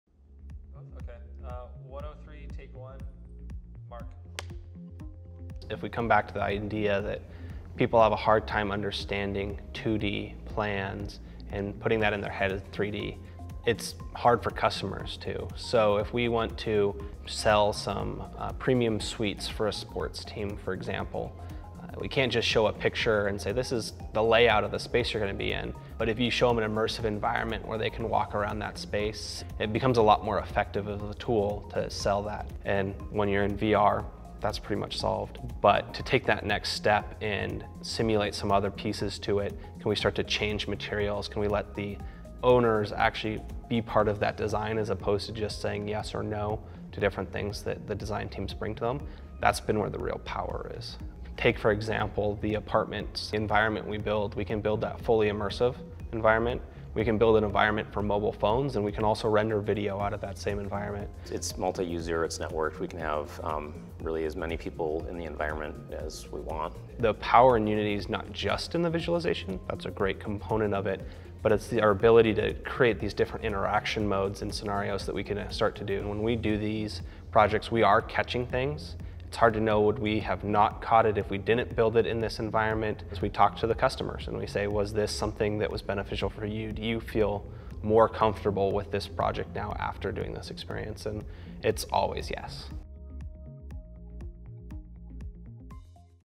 Unity for AEC: Better building designs and lower costs
Learn how Mortenson lessens the high costs and immense risks of construction projects. By simulating new building designs – and showcasing other technology – in interactive 3D spaces, their customers can fully visualize and engage with new virtual realities on their projects before they begin. This means benefits like improved designs, faster iterations, higher quality, and lower costs.